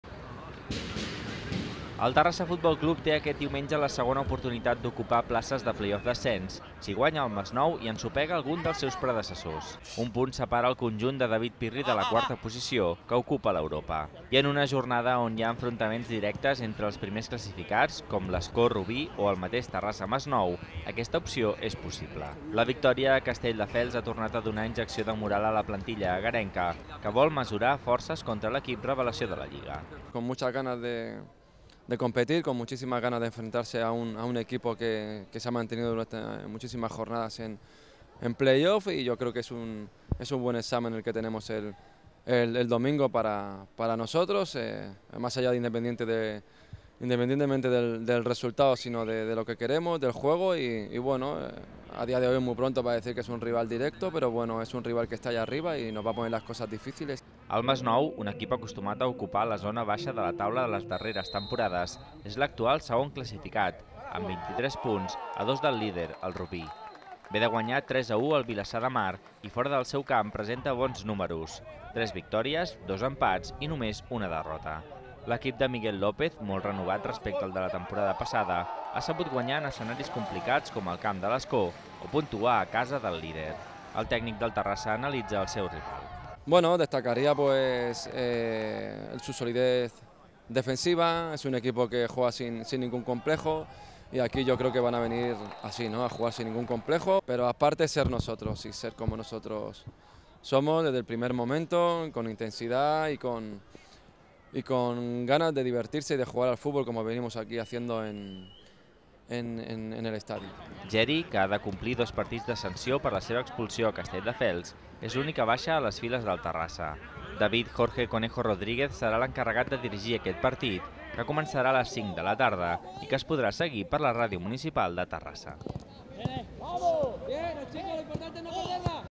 El Terrassa FC s'examina davant del segon classificat, el CD Masnou
El conjunt de David Pirri podria entrar en llocs de play-off d'ascens, si guanya i perd algun dels equips que el precedeixen. Geri, que ha de complir dos partits de sanció, és l'única baixa que tindrà David Pirri per aquest partit.